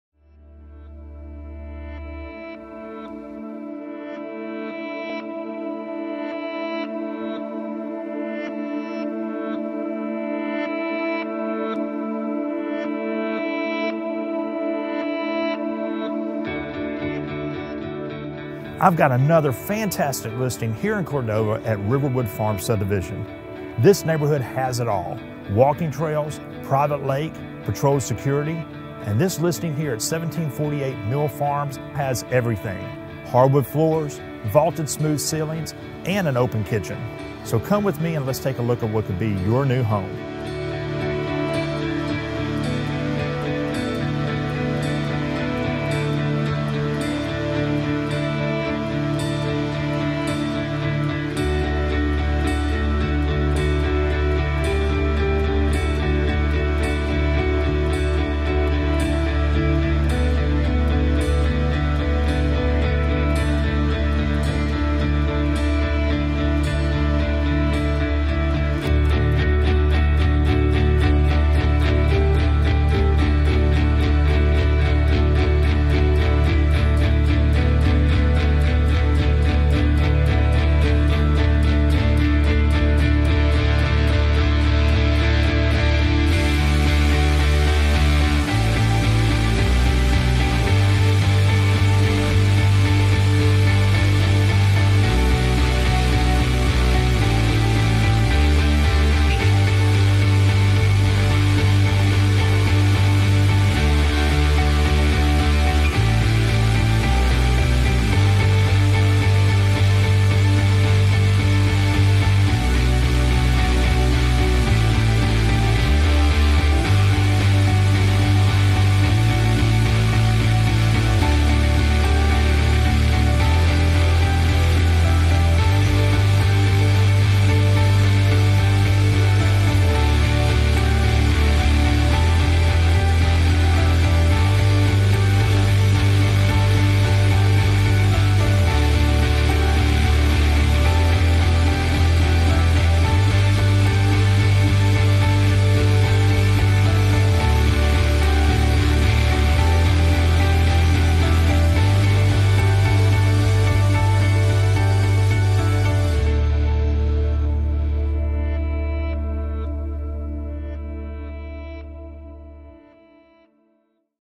Video Tour of 1748 Mill Farm Dr, Memphis, TN 38016
I have another beautiful listing at Riverwood Farms in Cordova, TN.  This home is located at 1748 Mill Farm Dr (MLS # 10025784) and looks like a show place.  Stunning cherry hardwood floors, smooth vaulted ceilings and an open floor plan.  Features an optional 4th bedroom or bonus room.  Spacious kitchen with updated appliances and a separate keeping room.  Landscaped yard with automatic irrigation system.  Enjoy the neighborhood lakes, park and walking trails.

Coldwell Banker Collins-Maury
TN Lic # 271807